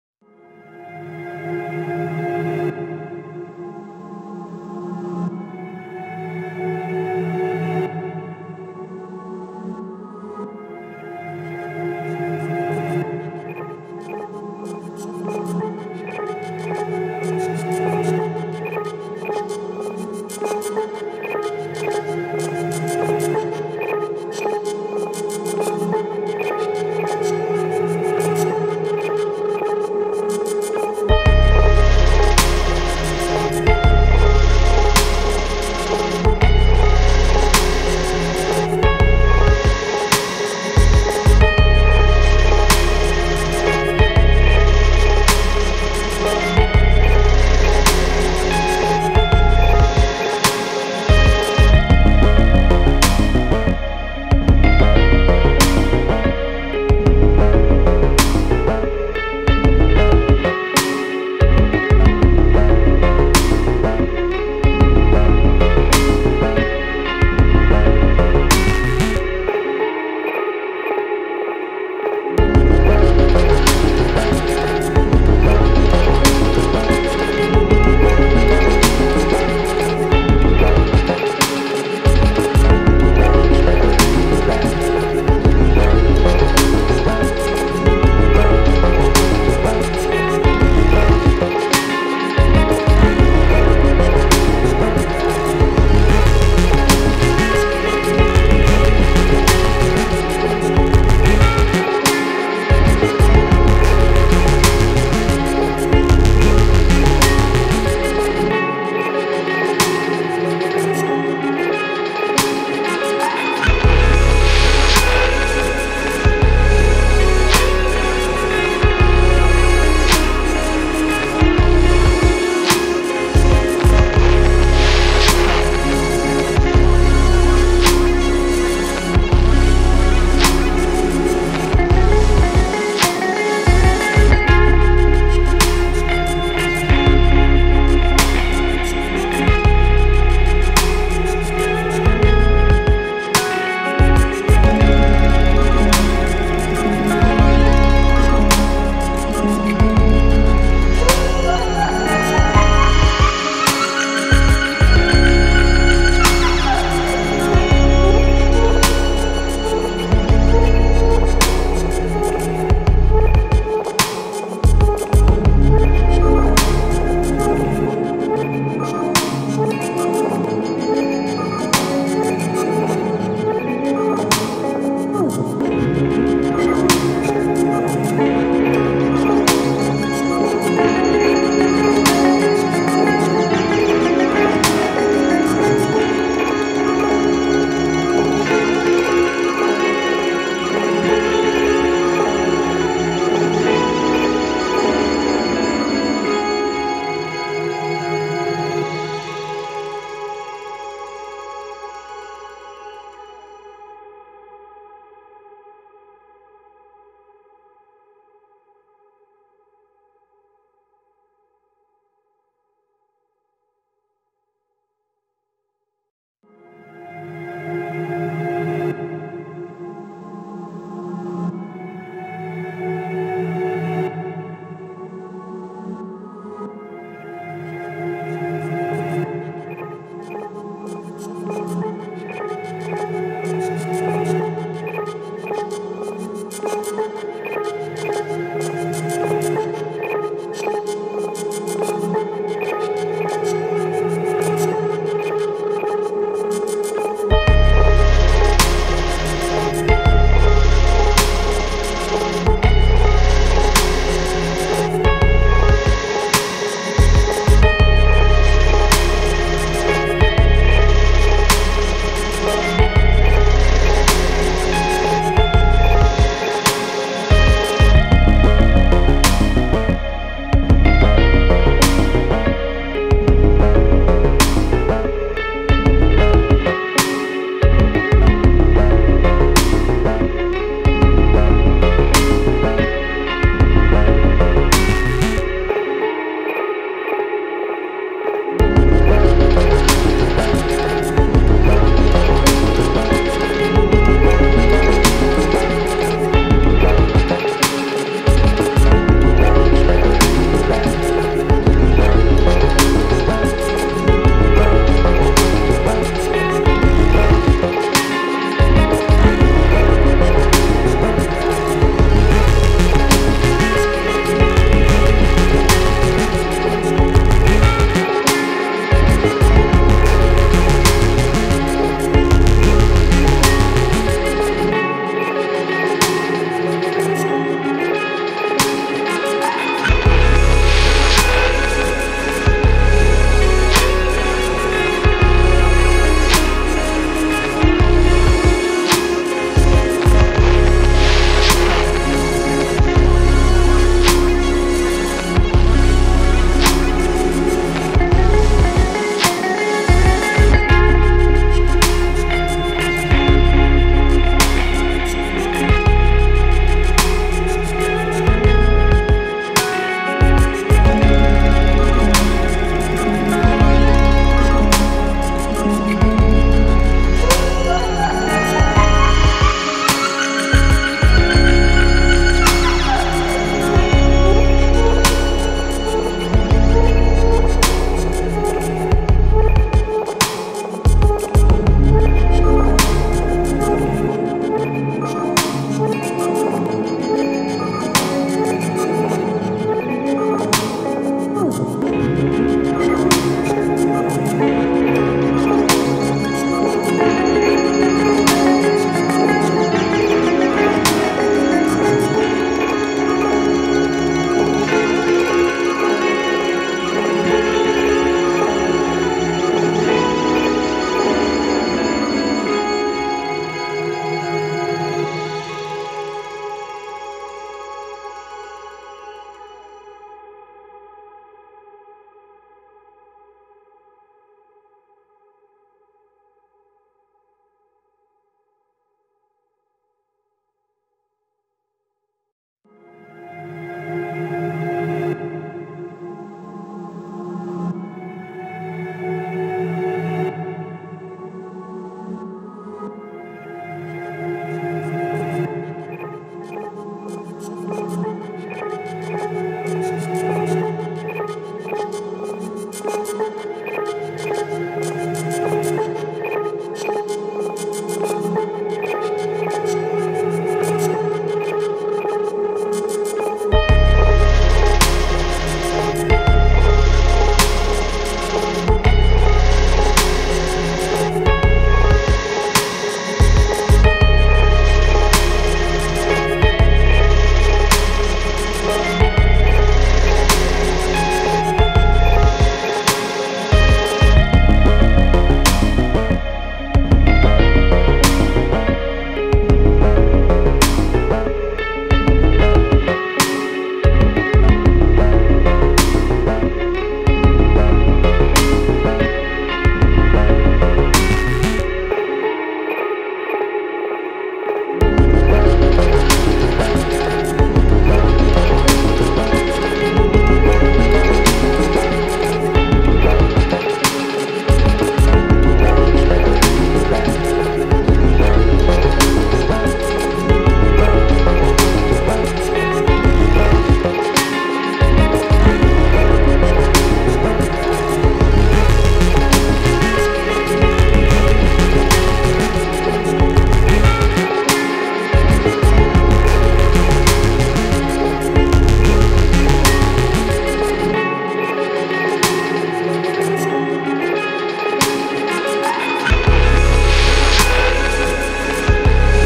Speed Art Poster #236 Ridge #9
Today I used a light blue color to contrast the dark and sharp geometric shapes.